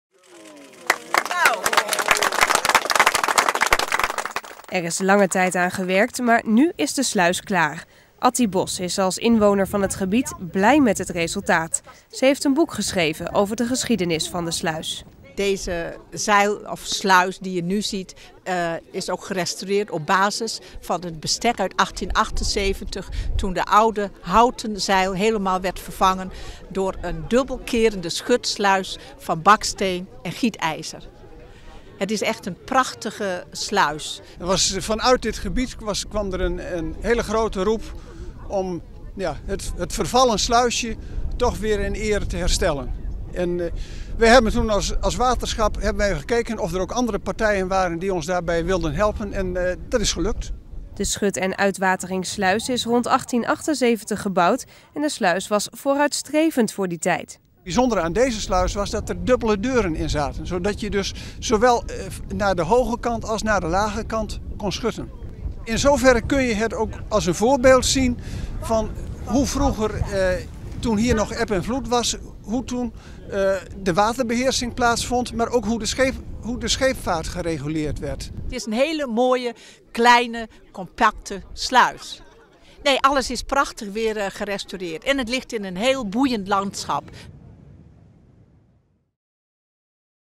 Wetsingersluis is weer in gebruik - RTV Noord
De gerenoveerde Wetsingersluis is vrijdag officieel in gebruik genomen.